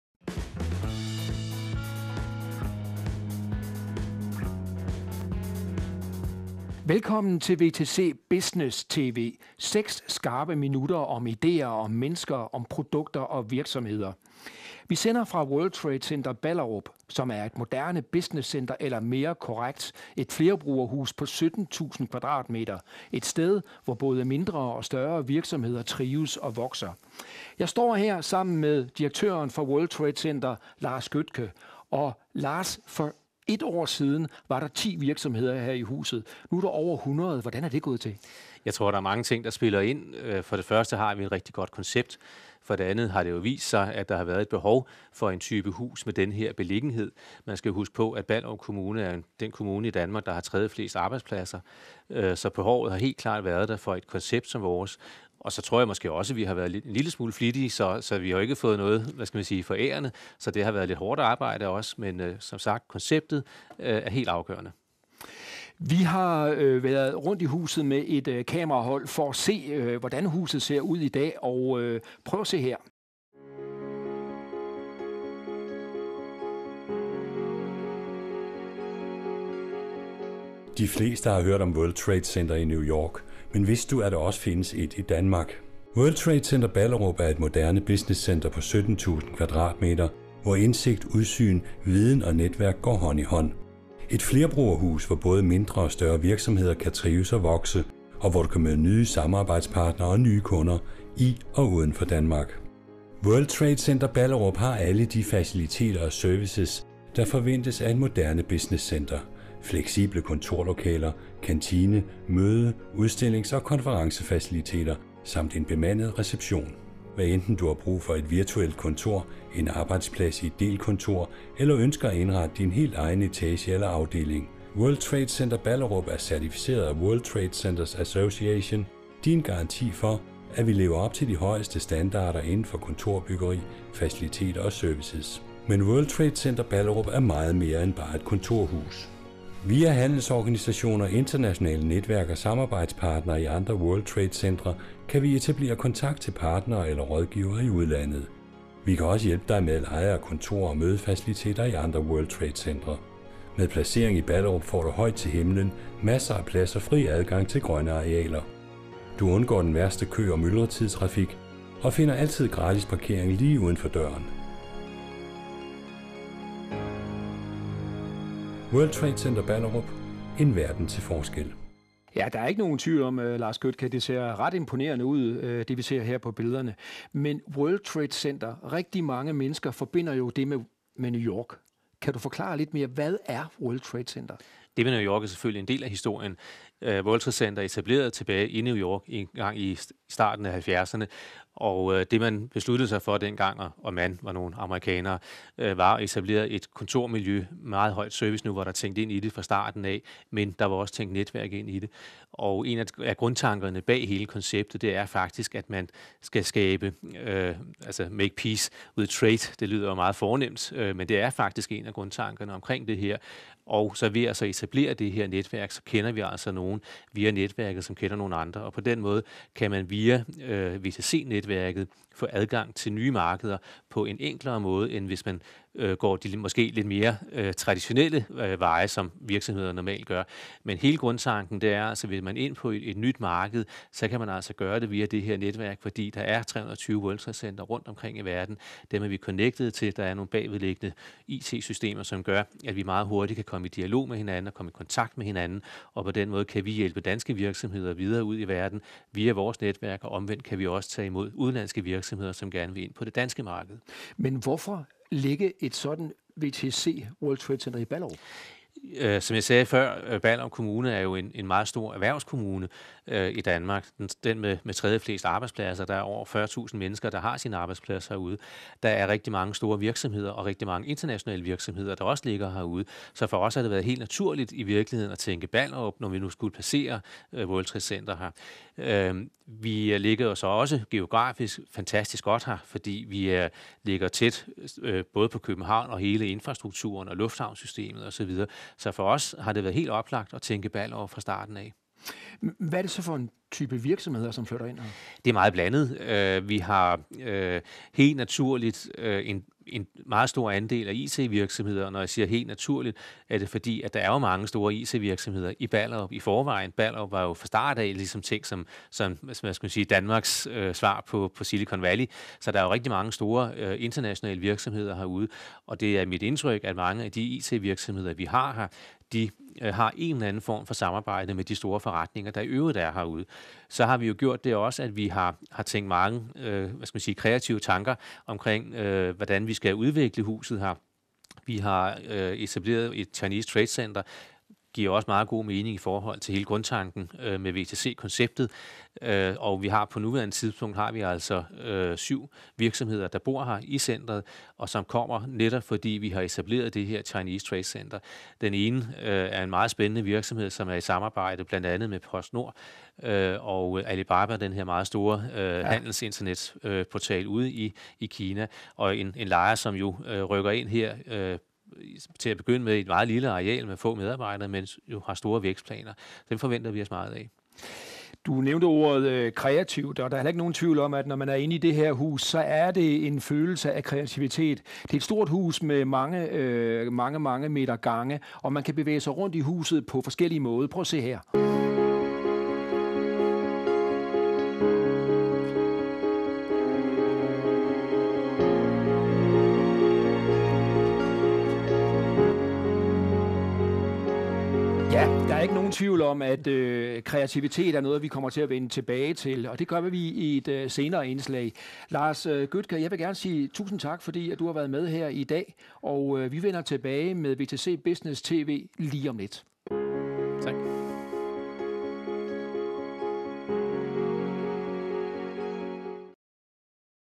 WTCBusinessTV EP01
Premiere på WTC Business TV.
6. minutter der stiller skarpt på ideer, mennesker, produkter og virksomheder. Vi sender fra World Trade Center Ballerup med direktør Lars Gøtke i studiet.

Vi taler denne gang med Lars Gøtke fra World Trade Center Ballerup

Produceret af Balorama Story Business